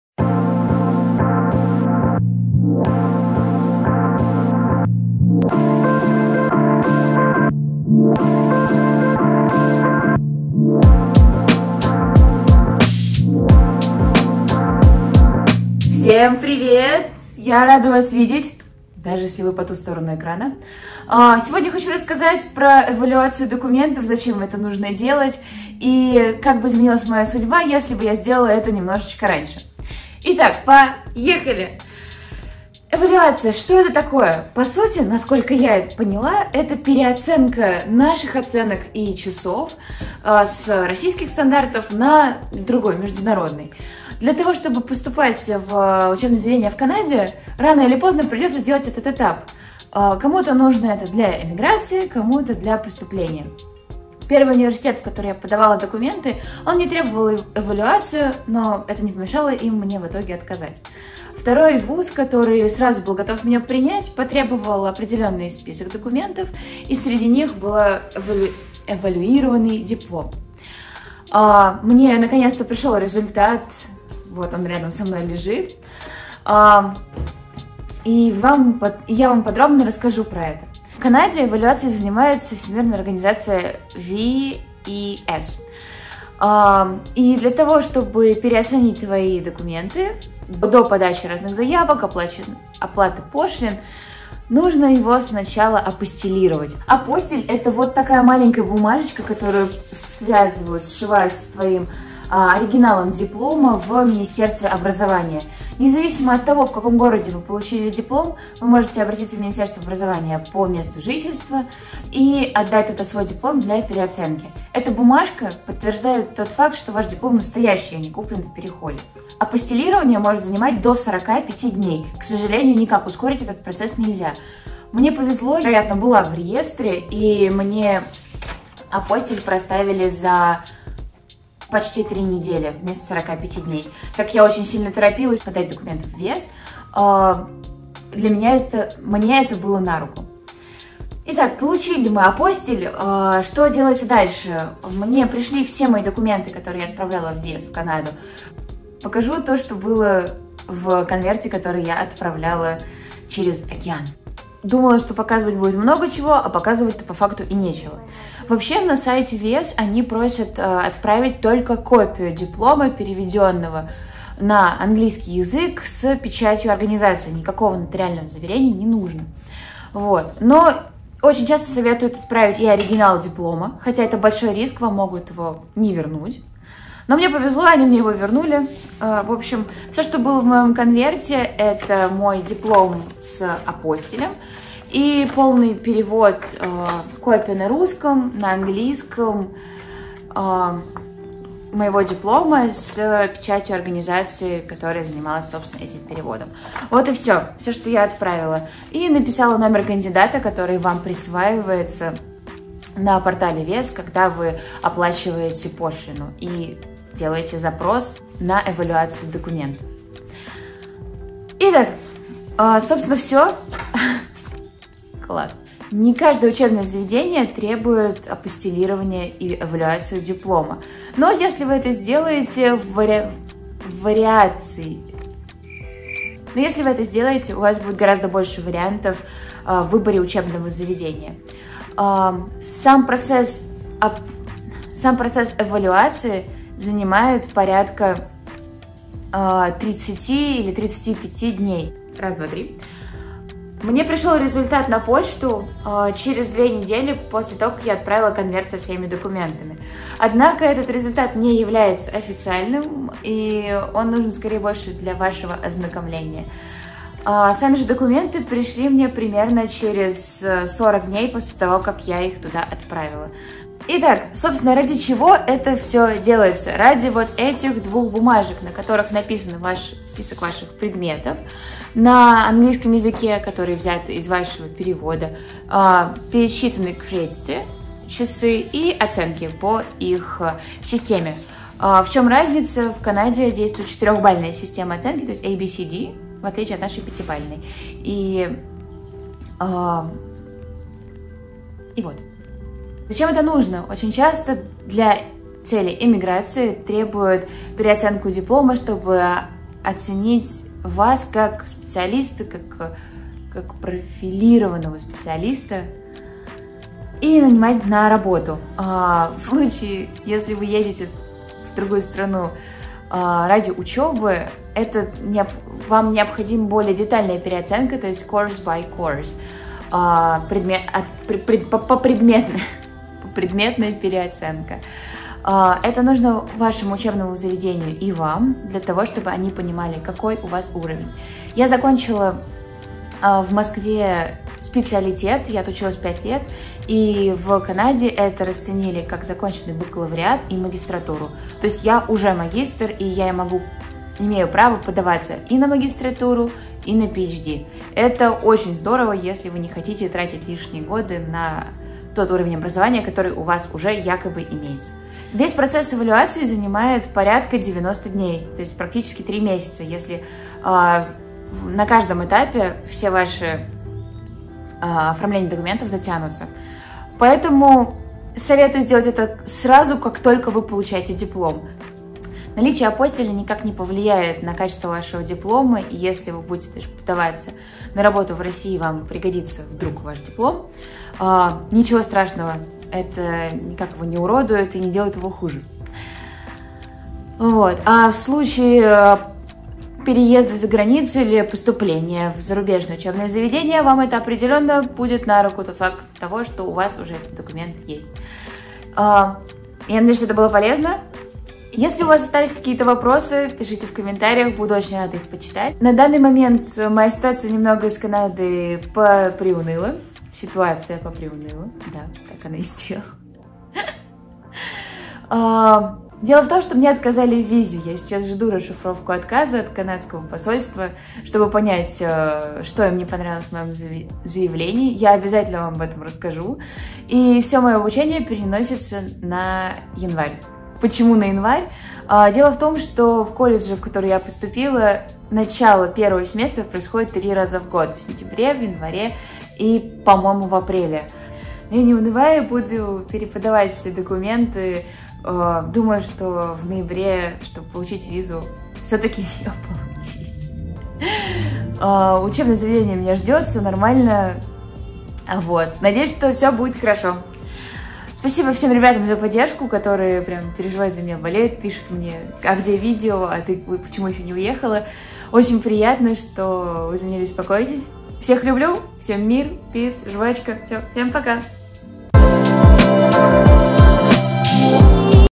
|WES CANADA| Если бы я сделала это раньше, то поступила в магистратуру в Канаде
Всем большой привет!

Если вы хотите поступать учиться за рубеж, вам рано или поздно придётся заняться вопросом эвалюации вашего диплома. В этом видео рассказываю про проставление апостиля и эвалюацию в  организации WES, которая занимается переоценкой документов для США и Канады.